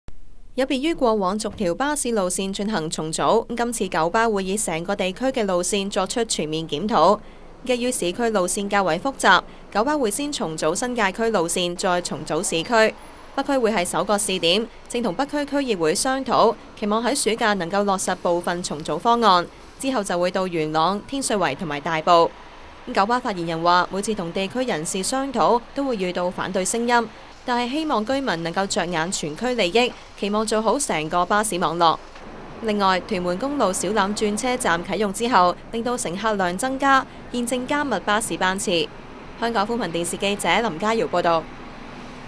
九巴重組路線北區先行
九巴正進行區域性巴士路線重組，北區會是第一個試點，正跟北區區議會商討重組方案，期望能在暑假落實。